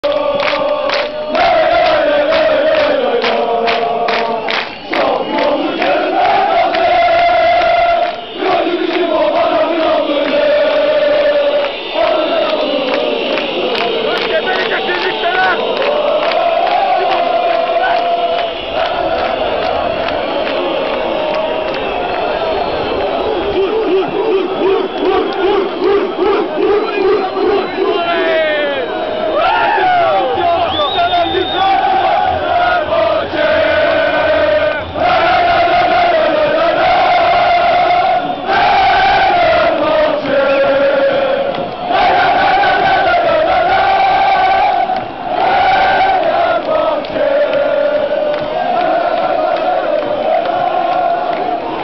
Galatasaray-Polis 1
Fener tribünden cekilen Galatasaray taraftari-Polis kavgasi! Galatasaray-Fenerbahce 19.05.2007